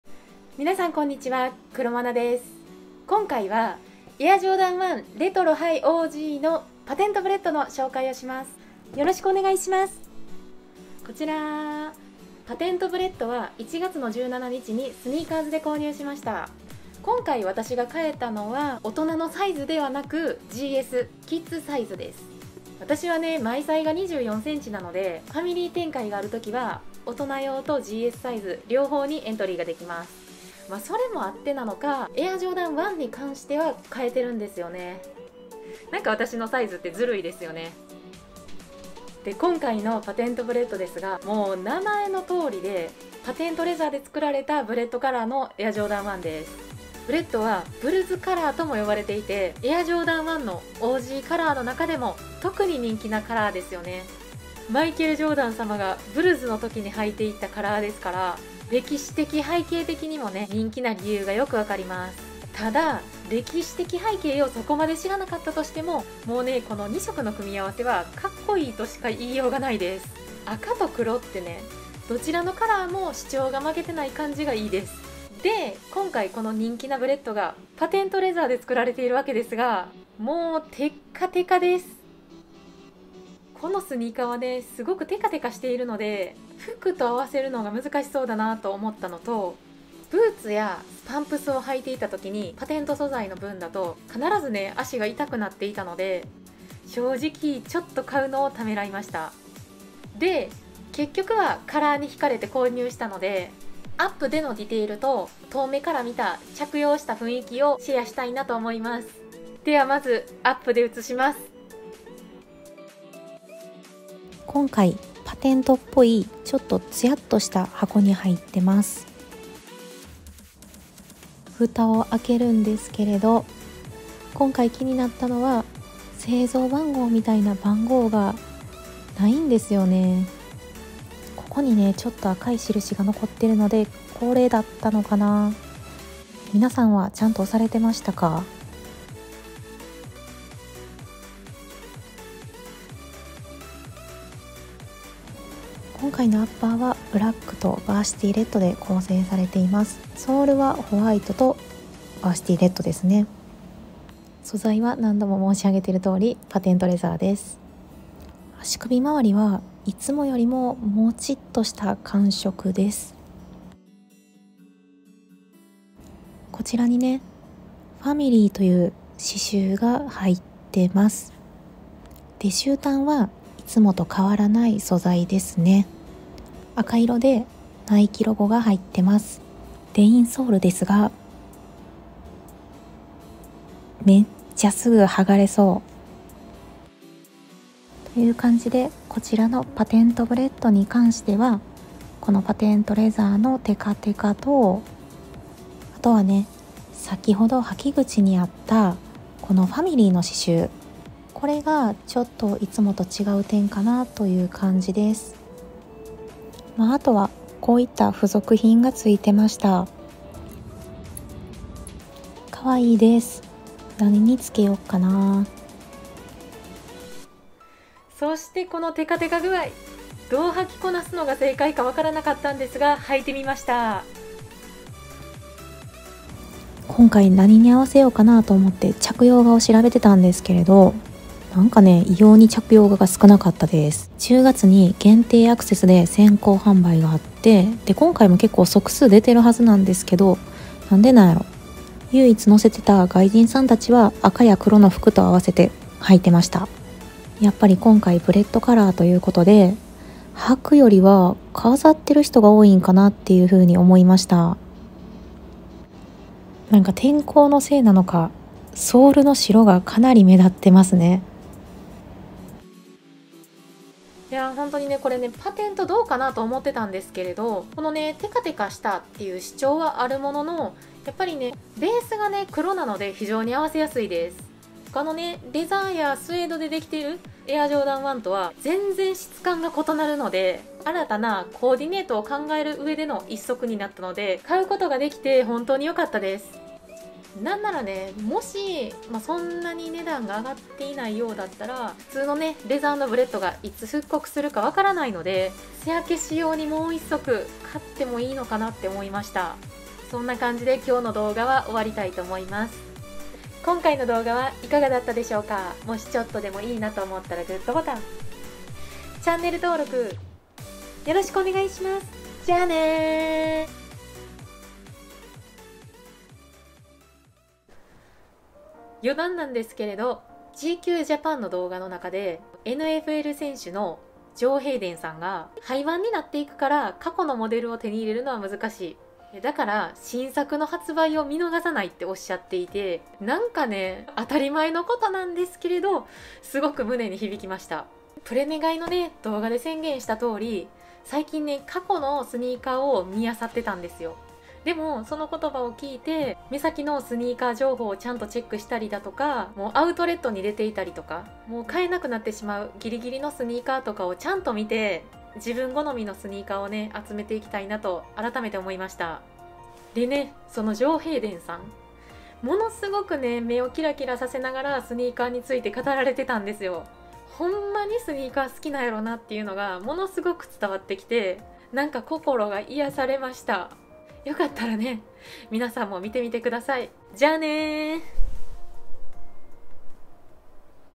AJ1 High OG "Patent Bred"早速履きました〜◎◎◎【パテントブレッド】
途中でめっちゃ大きい音のところがあってごめんなさい😭
音の調整がむずかしすぎます...😭😭💦

00:00〜 スタート
00:39〜 patent bredについて
01:53〜 スニーカーアップ
03:46〜 着画
05:18〜 余談

2022年1月17日発売

Nike GS Air Jordan 1 High OG "Patent Bred"
ナイキ GS エアジョーダン1 ハイ OG "パテント ブレッド "

購入を考えている方の参考にして頂ければと思います。